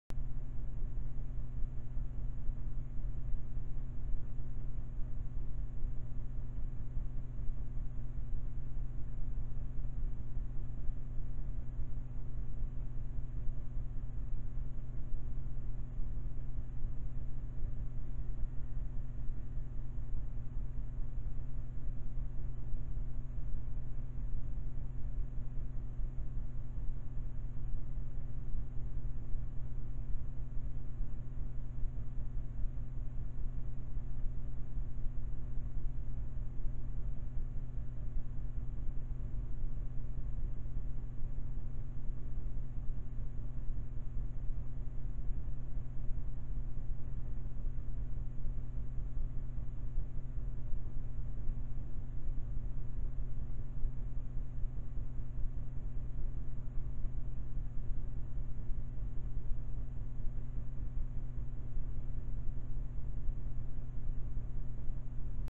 MAS201908035 BNSF 4277 ES44C4 northbound 22 Aug 2019 05 54 PM Edmond Oklahoma Ronald E Estes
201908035 BNSF 4277 ES44C4 northbound 22 Aug 2019 05:54 PM Edmond Oklahoma Ronald E Estes